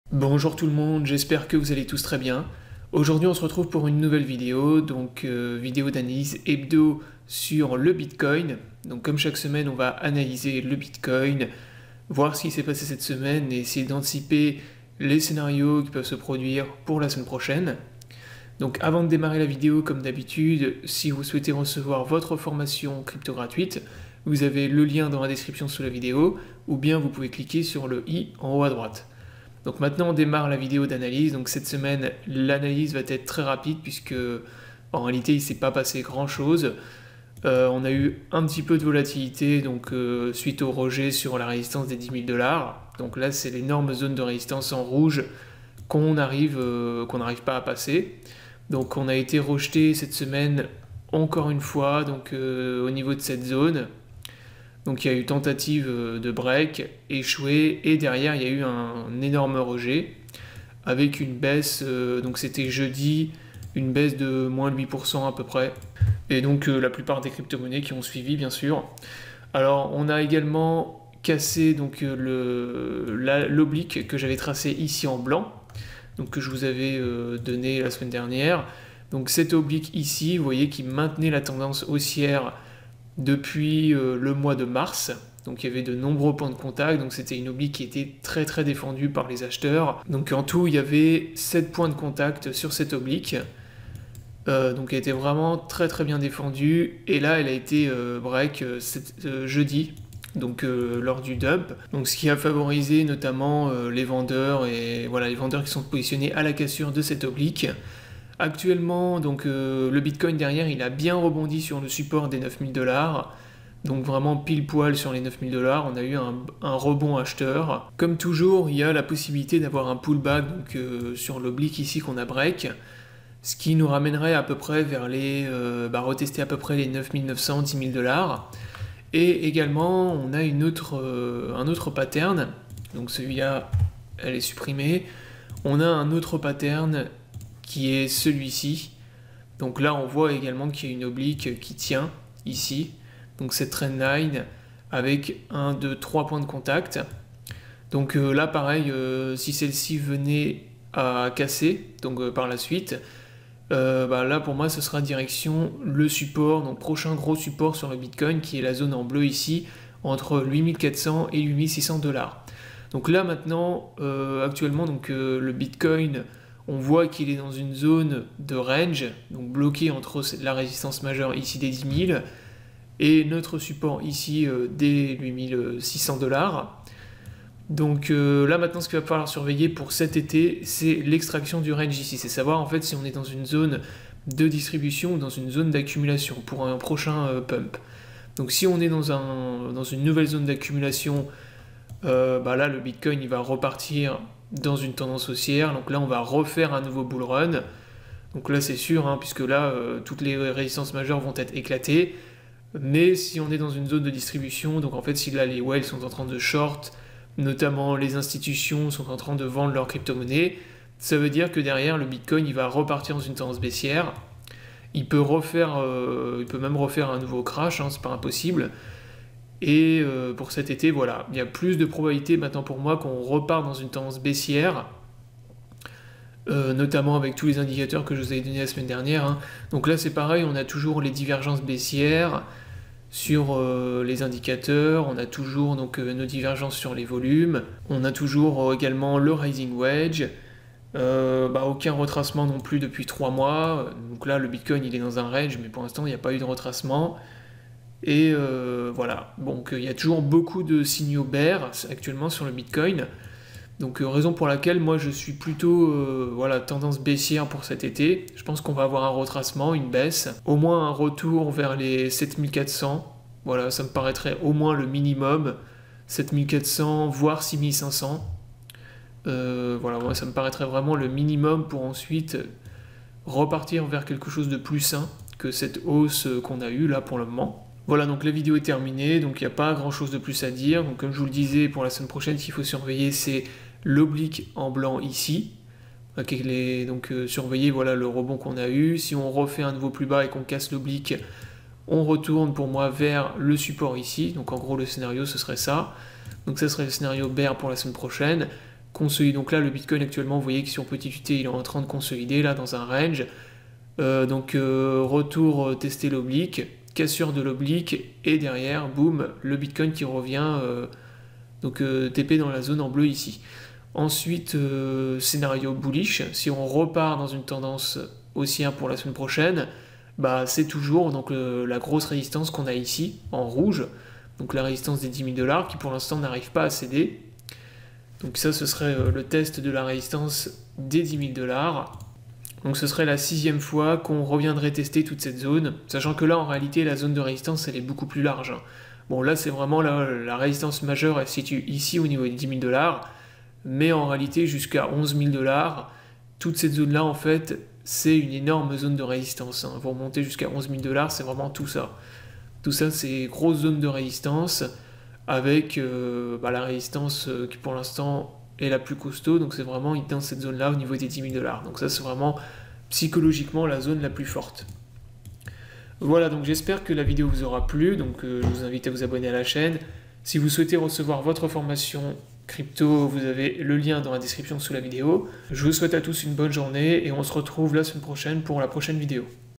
La chute du BITCOIN a commencé ?! Retour vers 8000$ ? Analyse Bitcoin FR
Découvrez comment investir avec l'objectif de générer un revenu régulier, CLIQUEZ
La chute du BITCOIN a commencé ?!
Un retour vers 8000$ est-il possible ? Découvrez nos analyses Bitcoin FR de cette semaine pour savoir si une chute du Bitcoin a commencé !

# chutebitcoincommence